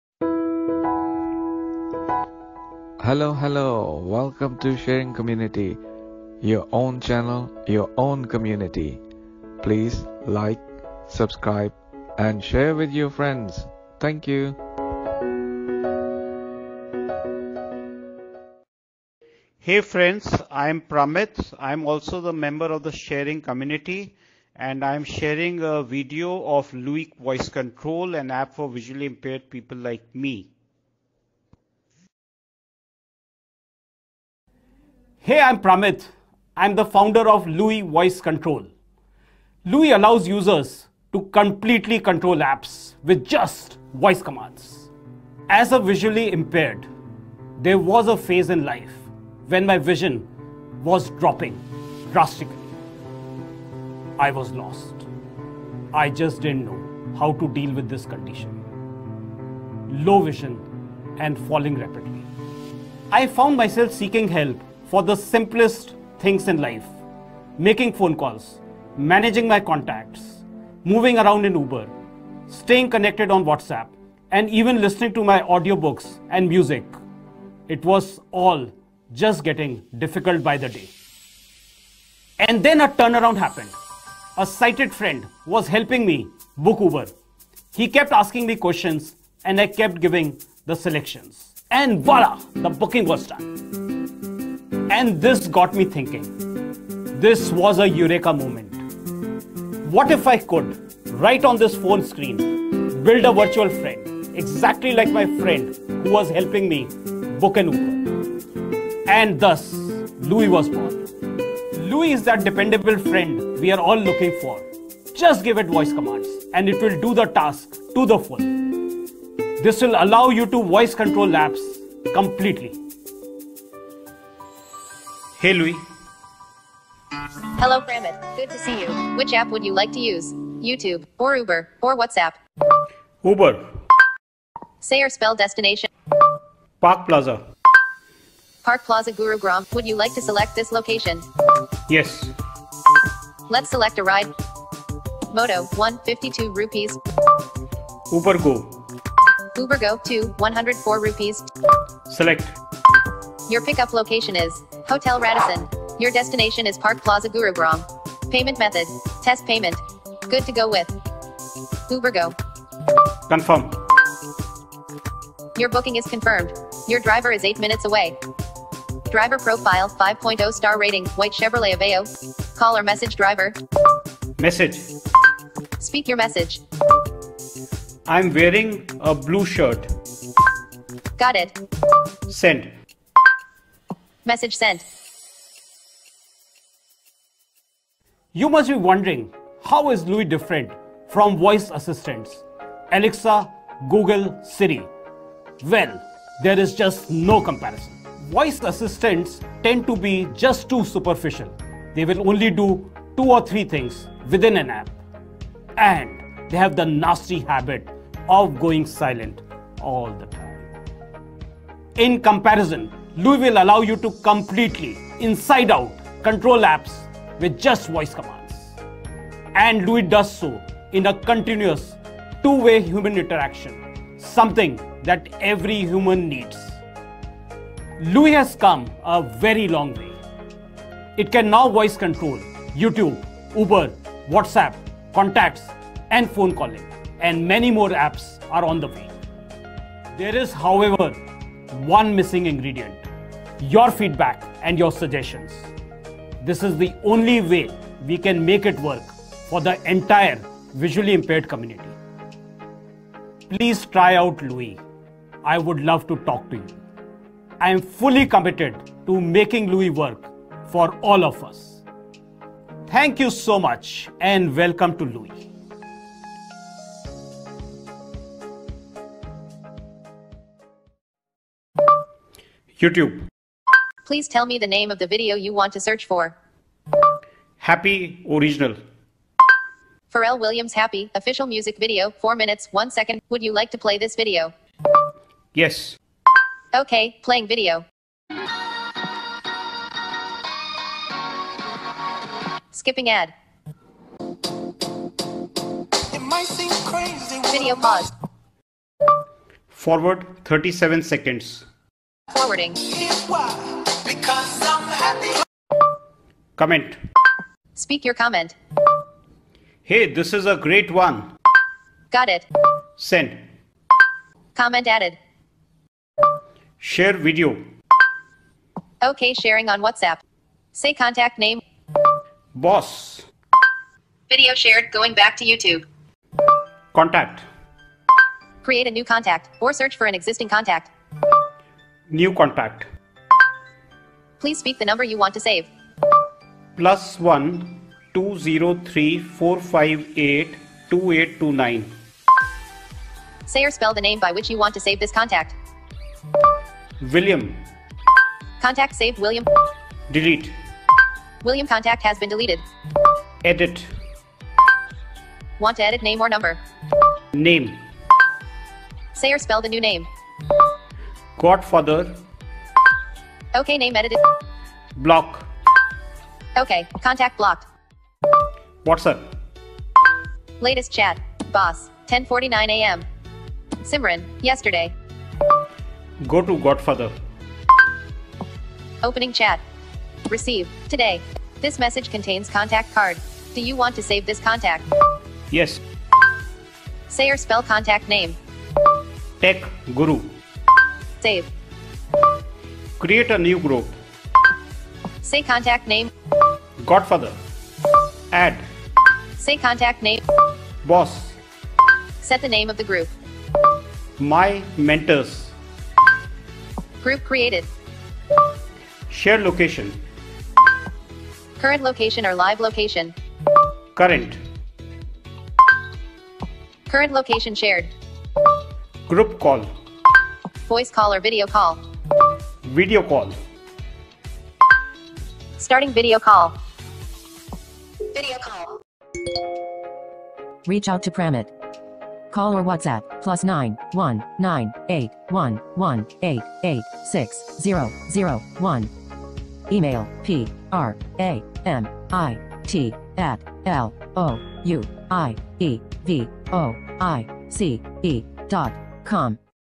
How to control your Apps like YouTube, WhatsApp, Uber etc.. With just your voice command short cut.
Welcome back to Sharing Community.

In this video, founder of Louie App, Mr. Pramit, is telling us more about the application, and demonstrating, how we can interact with just our voice and giving  instructions how can we use  this  in our daily life, such as in YouTube, Facebook, WhatsApp, Uber, phone contacts, text,  call and many more.
Also why it's different to Siri, Google and Alexa.